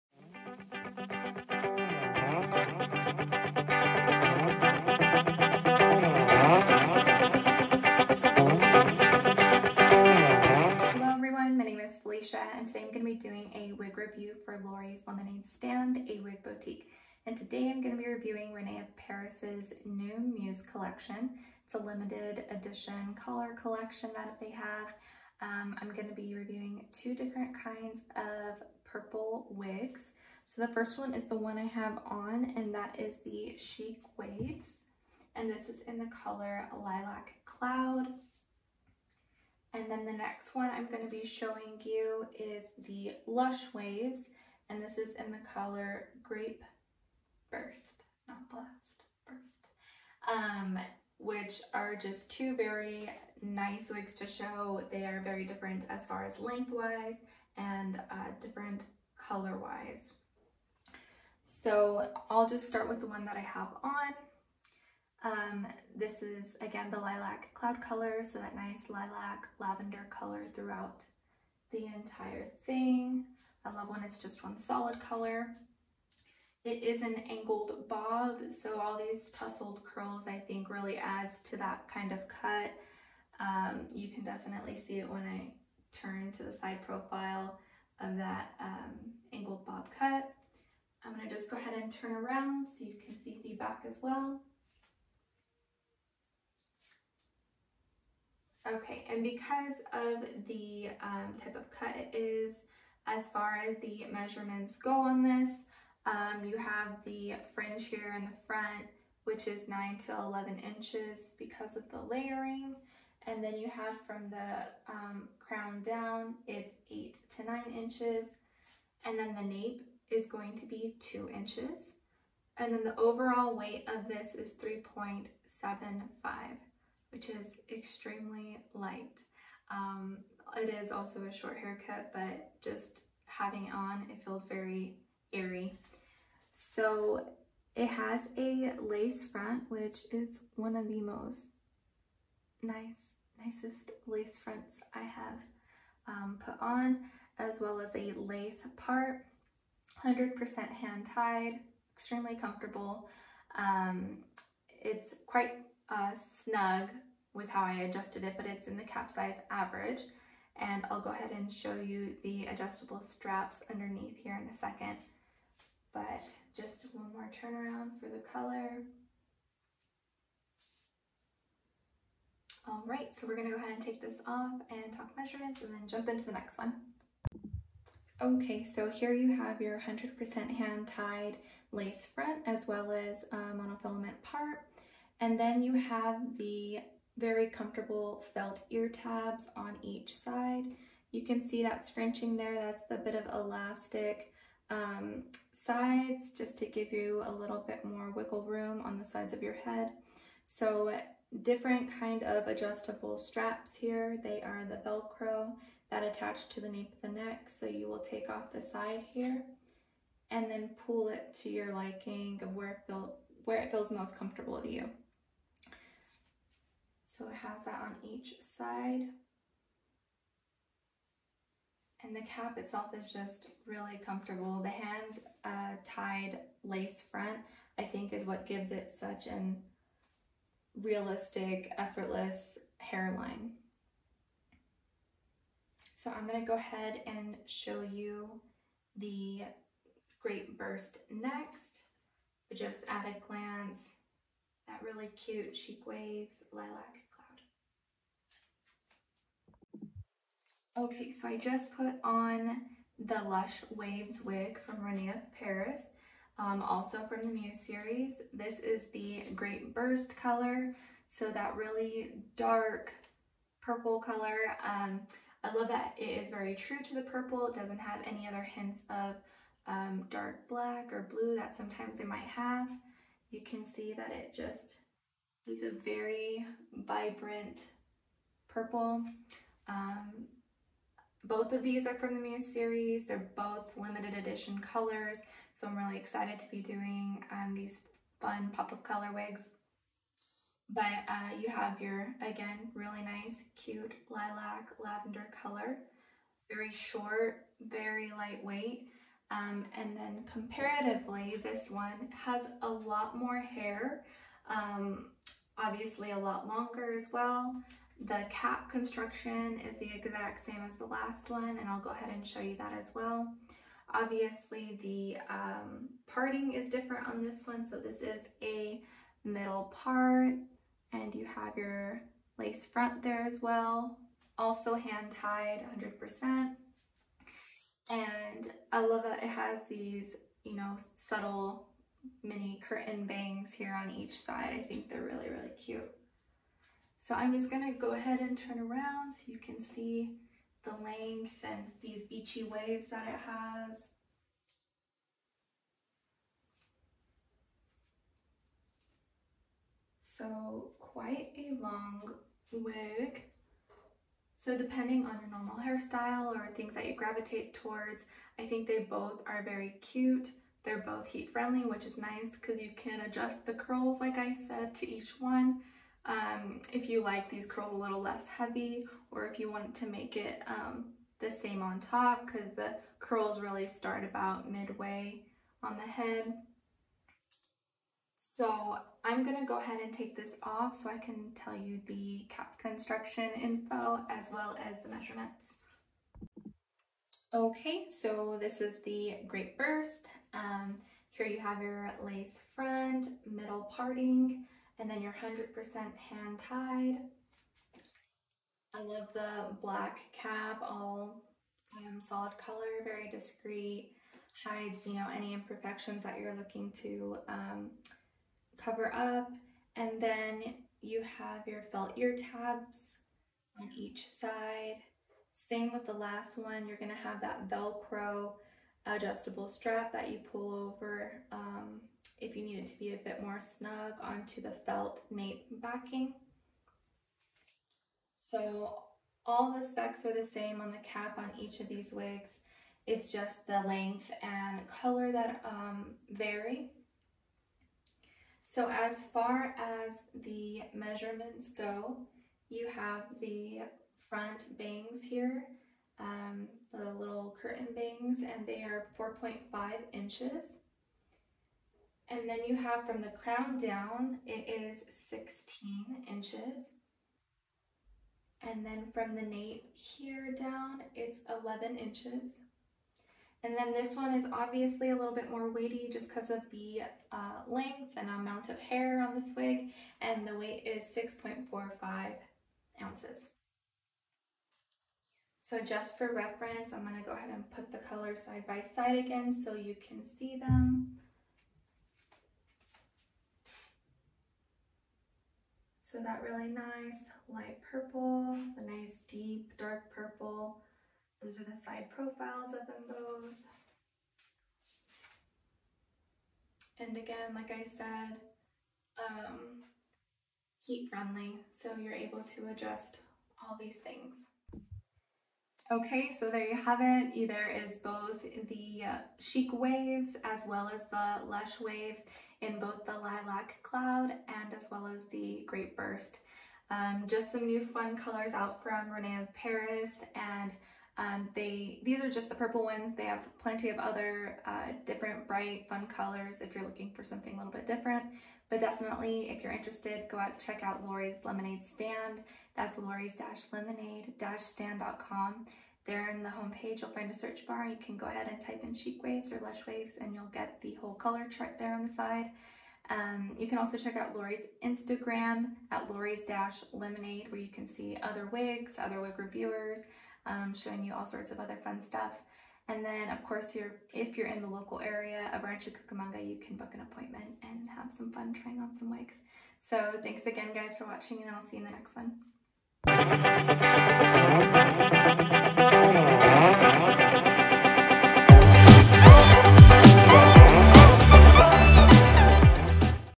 WIG REVIEW: Chic Wavez vs. Lush Wavez Muse Collection by Rene of Paris
Chic Wavez - Lilac Cloud
Lush Wavez - Grape Burst

Available at Lori's Lemonade Stand where we want to help you turn your lemons into lemonade!

13980 Guidera Drive
Rancho Cucamonga, CA